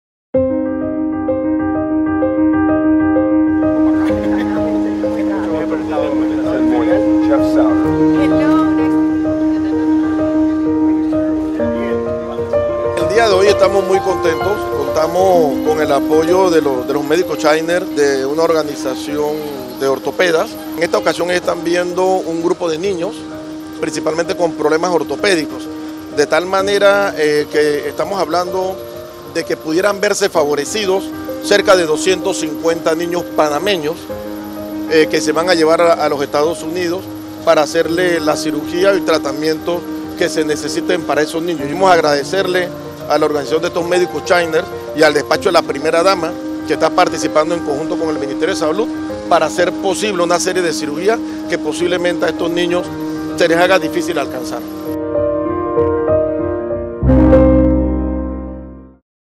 Visita de la Primera Dama Yasmin de Cortizo a nuestra clínica de Ortopedia 2020.
Visita de la primera dama a nuestra clínica anual de Ortopedia con médicos provenientes de Shriners Hospitals for children para la evaluación de los pacientes y verificación de nuevos casos.